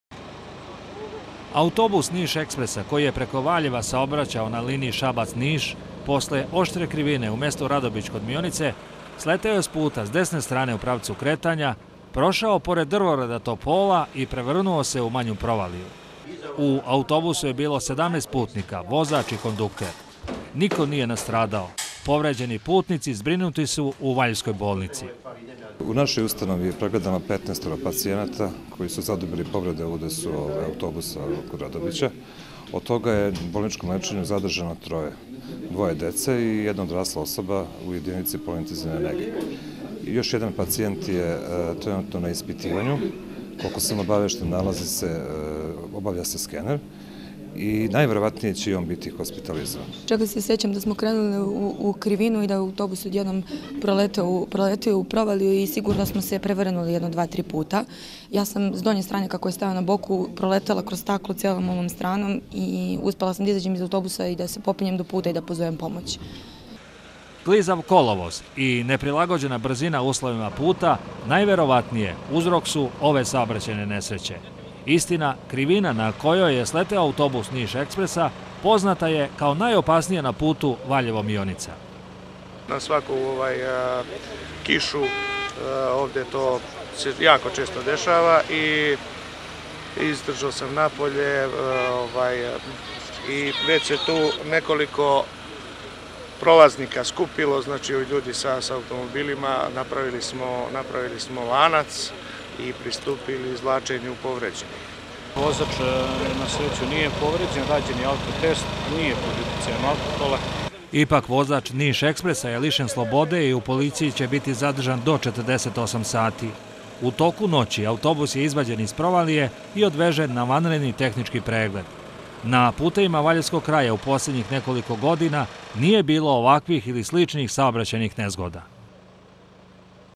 Prevrnuo se autobus kod Mionice, 17 povređenih
Teže povređeno sedam, a lakše deset putnika kada se autobus "Niš-ekspresa" prevrnuo u mestu Radobić kod Mionice. Niko nije životno ugrožen. Vozaču određeno zadržavnje do 48 sati. Uzrok nesreće najverovatnije je klizav kolovoz i neprilagođena brzina uslovima puta. Copyright : Javni Servis Srbije (RTS)